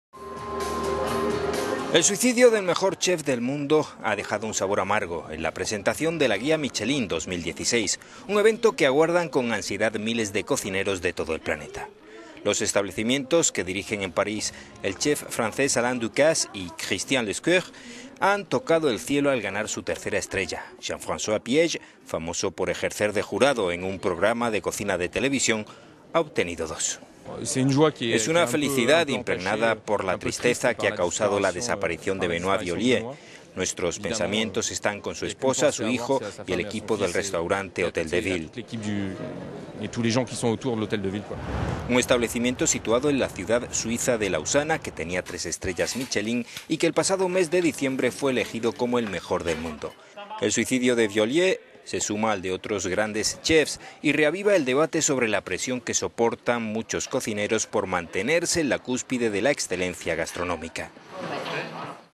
El suicido del mejor chef del mundo deja un sabor amargo en la presentación de la guía Michelín…
El suicido del mejor chef del mundo ha dejado un sabor amargo en la presentación de la guía Michelín de 2016, un evento que aguardan con ansiedad miles de cocineros de todo el planeta. Los establecimientos que dirigen en París el chef francés Alain Ducasse y Christian Le Squer han tocado el cielo al ganar su tercera estrella. Jean-François Piège, famoso por ejercer de jurado en un programa de cocina de televisión, ha obtenido dos:

"El ambiente está impregnado por la tristeza que ha causado la d…